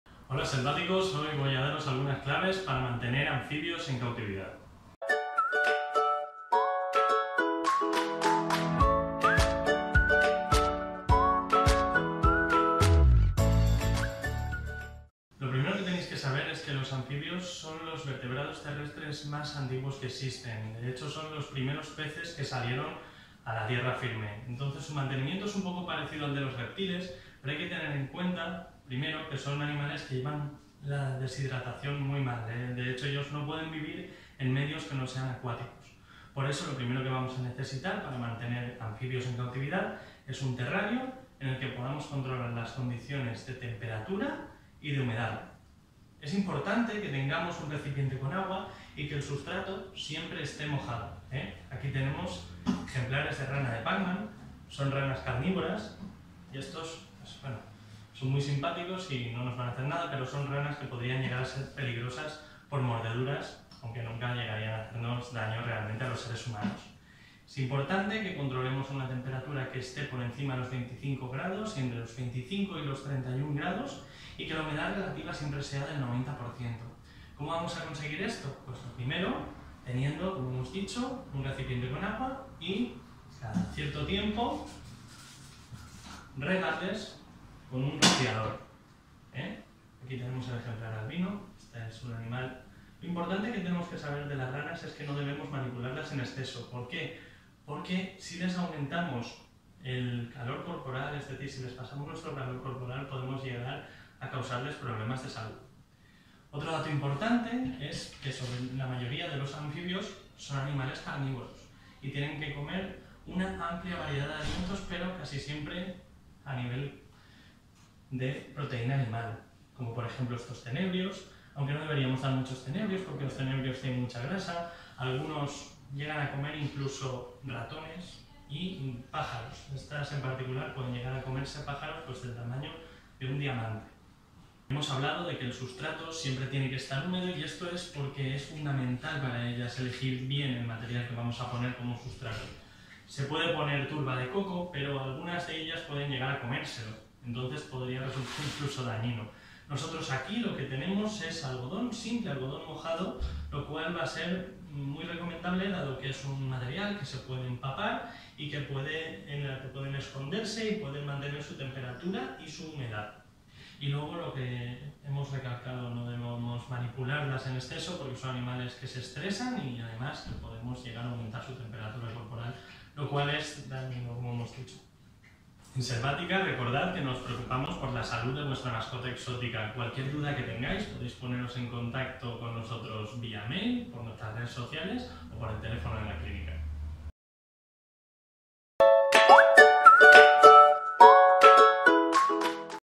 Cómo mantener anfibios en cautividad
Hola Selväticos!! En este nuevo vídeo Juan nos explica que cuidados tenemos que tener con nuestros anfibios, alimentación,sustrato,humedad etc... En el vídeo aparecen unas ranas muy curiosas ya que son carnívoras.